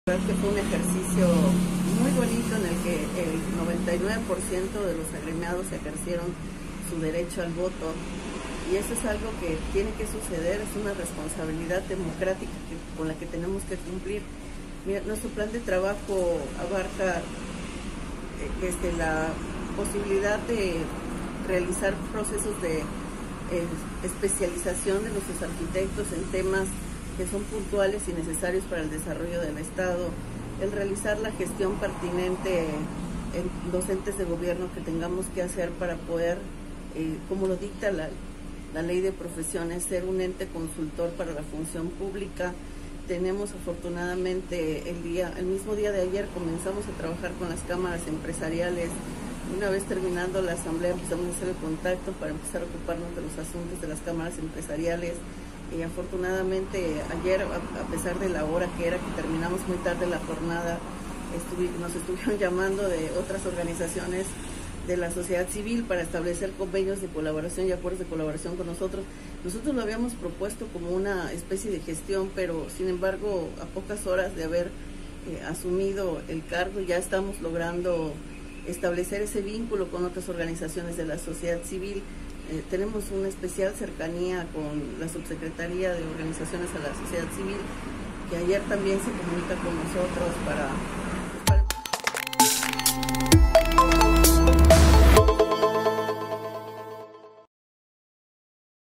KARLA SÁNCHEZ RINDE PROTESTA COMO NUEVA PRESIDENTA DEL COLEGIO DE ARQUITECTOS
Quinta mujer en ocupar el cargo y la primera con discapacidad en México, rindió protesta como presidenta del Colegio de Arquitectos de Campeche, Karla Gelisle Sánchez Sosa, asumió está responsabilidad enfocada en impulsar proyectos que fortalezcan el Estado.